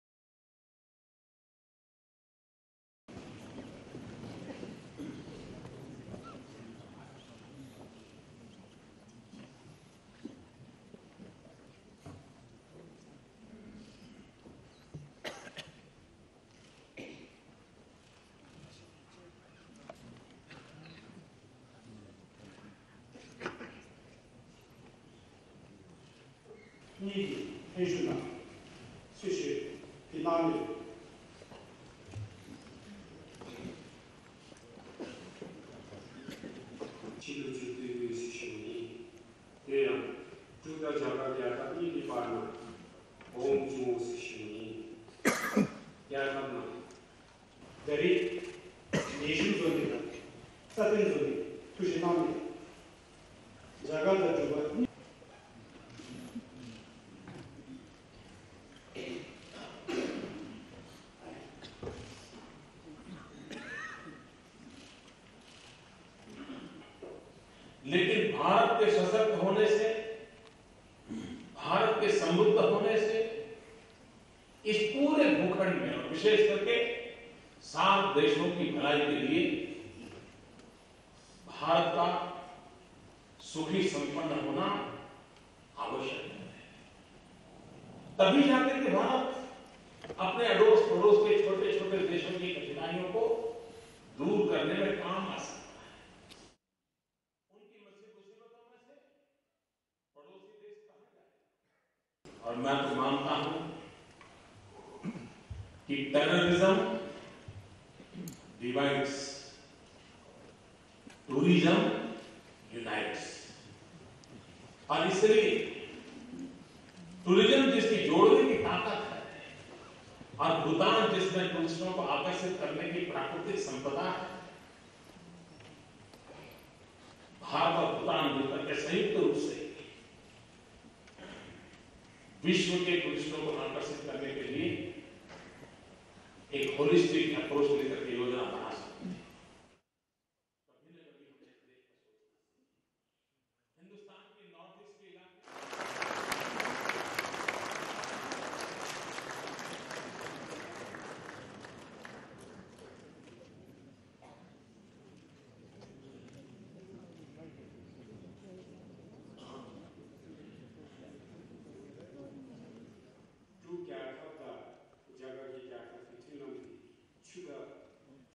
Modi says strong, prosperous India beneficial for Bhutan, SAARC nations
Prime Minister Narendra Modi says India's prosperity is significant and in the interest of Bhutan and SAARC nations, adding that his government is committed to make stronger relationship between the two nations.